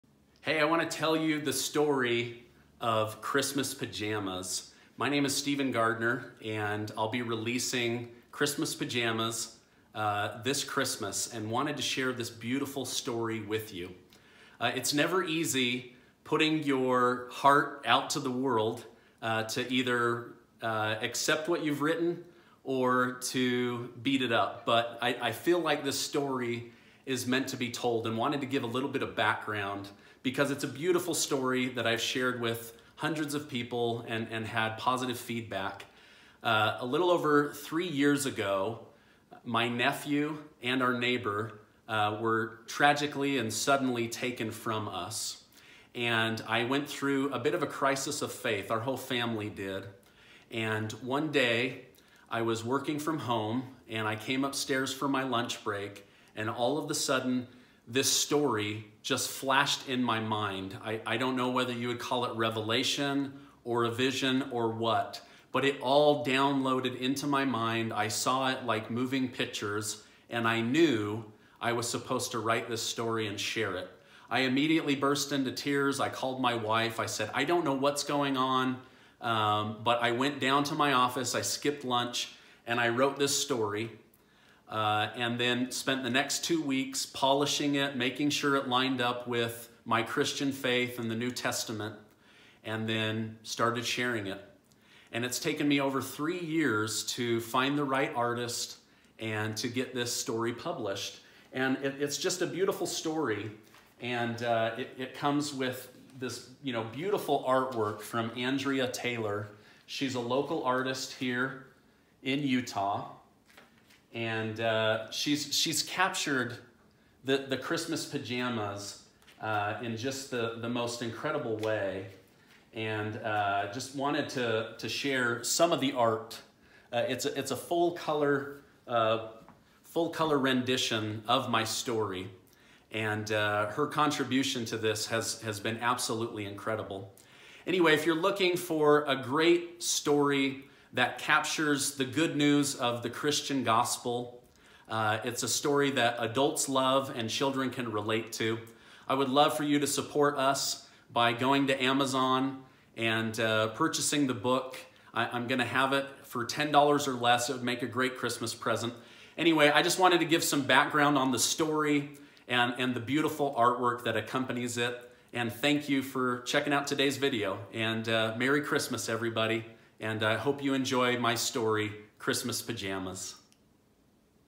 Christmas Pajamas
If you've ever lost a child or a loved one, you might have asked yourself why? Or where is this loved one now? What if you could visit heaven for a night and tuck your child in one last time.

After losing our nephew and neighbor tragically and suddenly, our family went through a crisis of faith. One day while walking to lunch I suddenly burst into tears as I saw a story play in my mind. What I saw that day is what I have written in Christmas Pajamas.

It has taken me over 3 years to find the right artist and get this story published. It's a beautiful Christian story that adults will love and children will be able to understand.

Part of the proceeds from the book will be donated to the family that lost their son. They will use that money to create a Festival of Trees tree to be purchased for a large amount of money. All that money will be donated to Primary Children's Hospital.

I hope you enjoy the story. I'm not sure why this story came to me, but I've written it and now want the world to have access to it. I give God the credit and thank you for the experience.